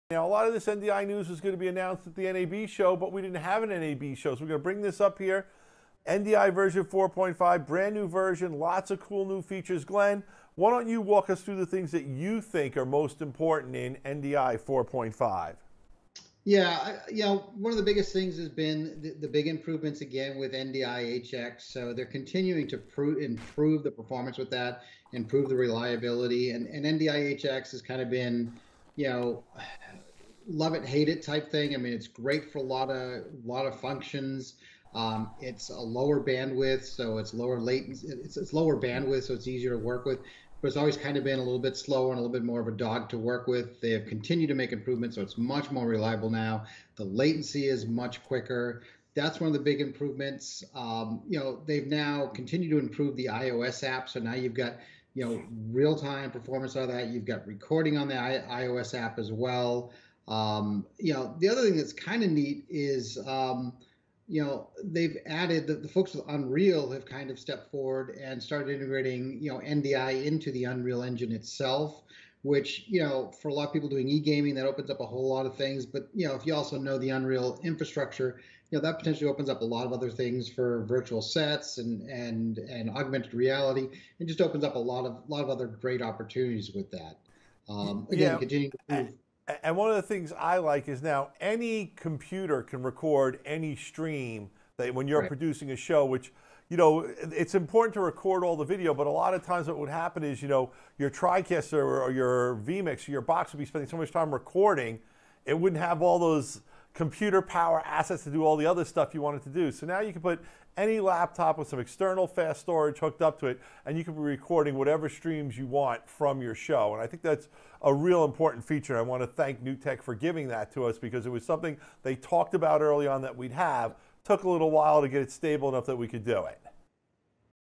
Videoguys LIVE Introducing NDI 4.5
The NDI 4.5 is here with real-time iOS support, NDI | HX improvements and lower latency, unlimited IP recordings, and NDI SDK for unreal engines.